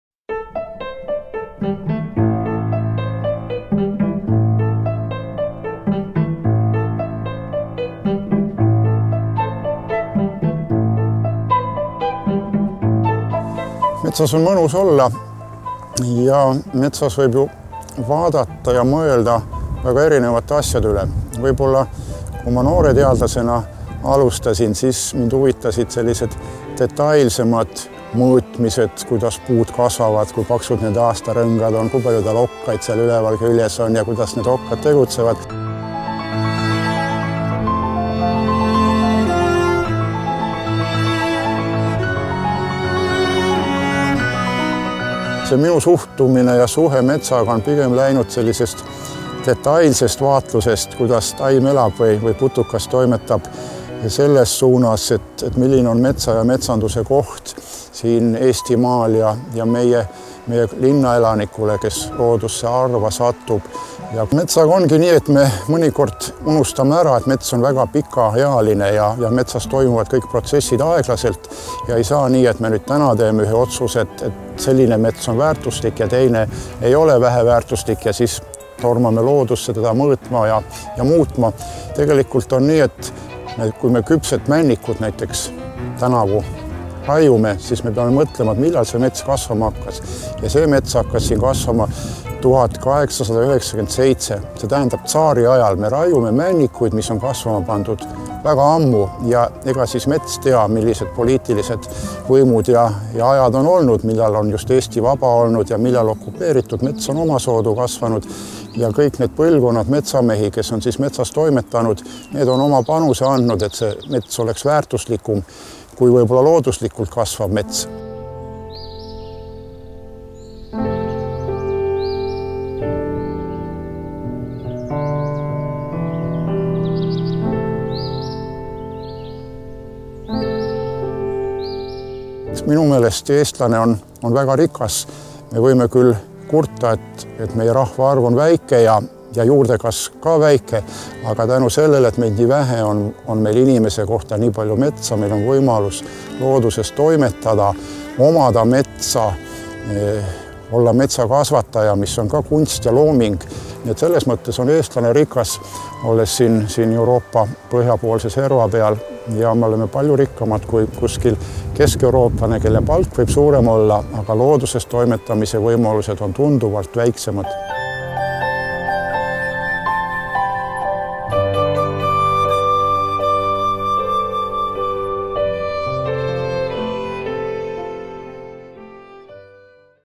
Aitäh, mets. Metsateadlane Hardi.
Eesti metsad annavad tööd enam kui 30 000 inimesele. Sellest koostööst räägib oma seisukohalt Eesti Maaülikooli professor Hardi Tullus.